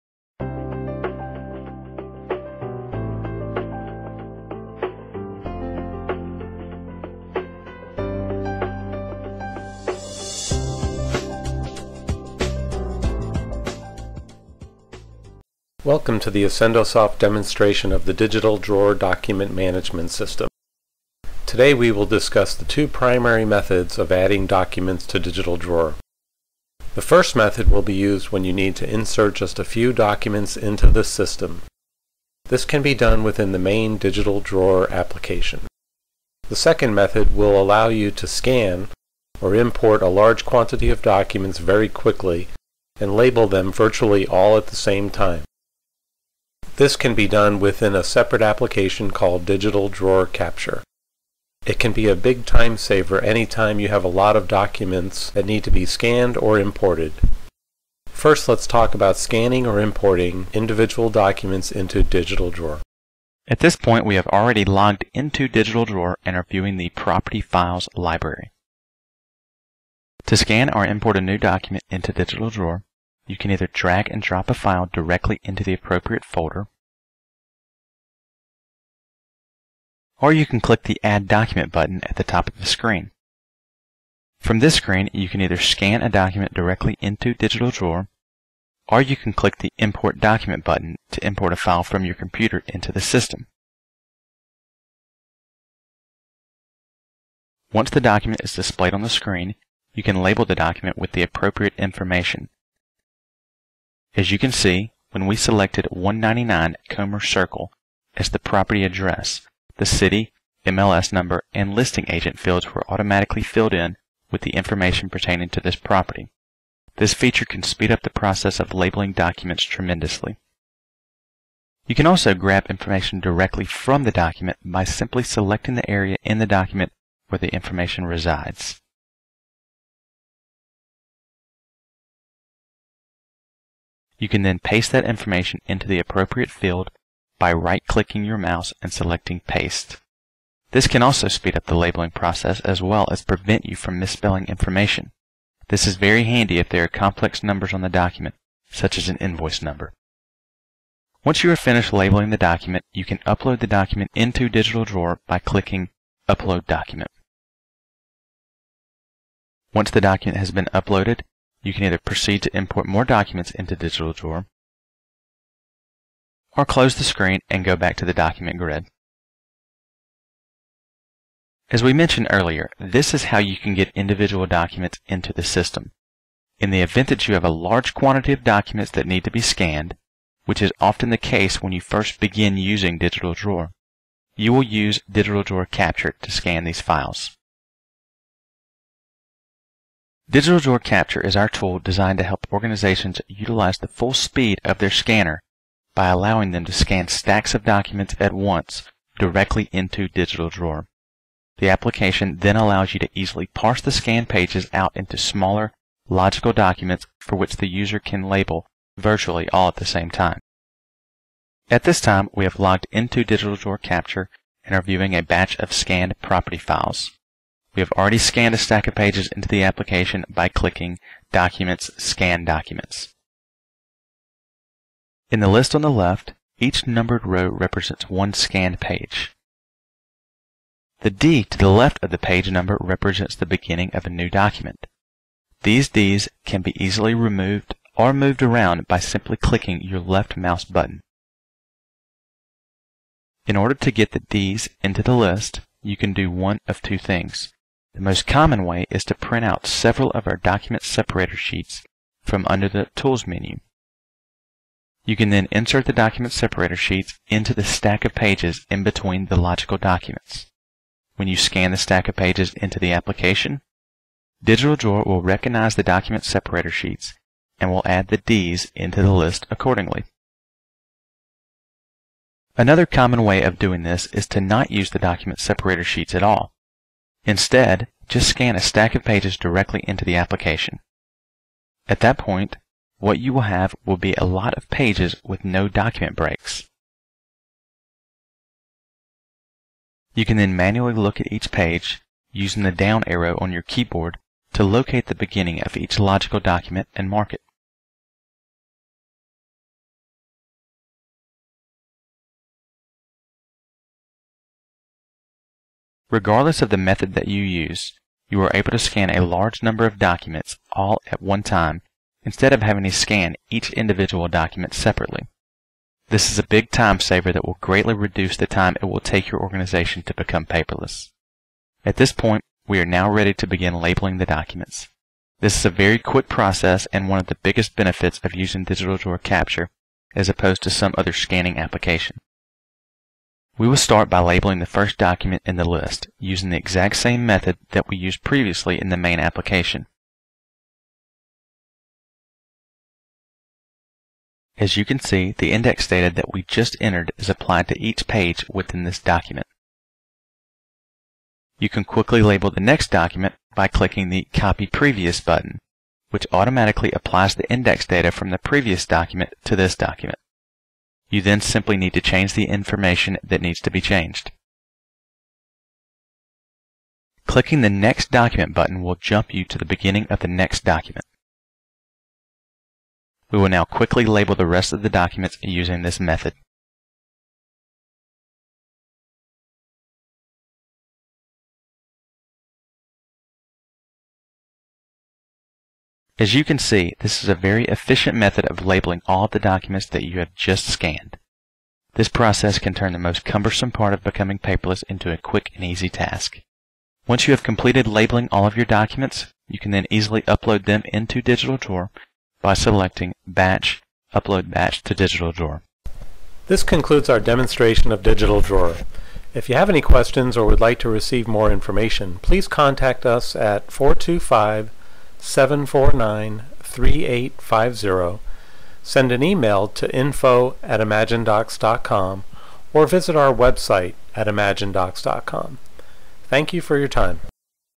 Electronic Document Management System - Scanning Real Estate Records
Short video on how to upload real estate records to DigitalDrawer.